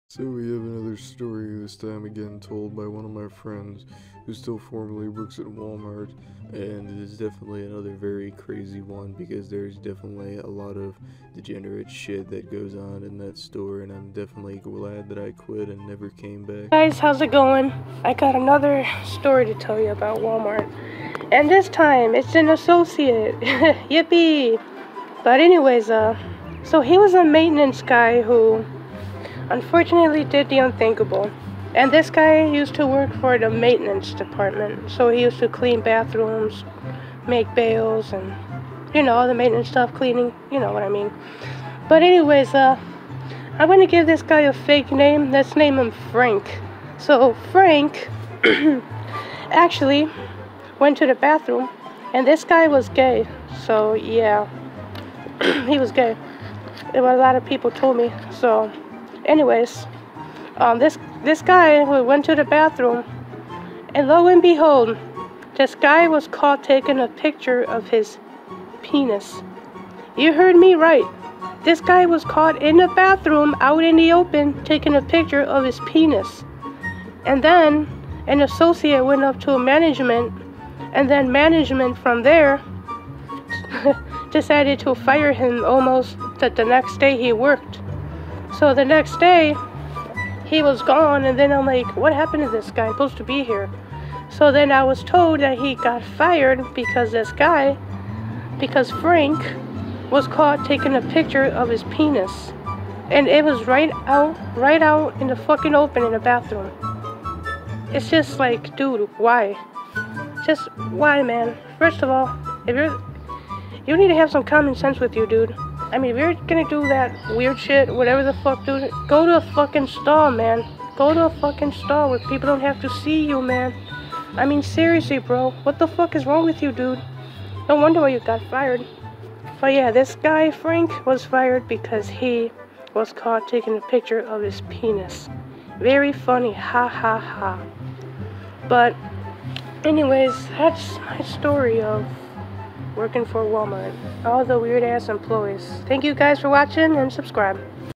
Guy Was Caught Taking A Picture Of His Dick At Walmart
Another story told by my friend who works at Walmart. This time about how a worker thought it was a good idea to snap a picture of his cock in the bathroom. Not even in a stall or anything just out in the open.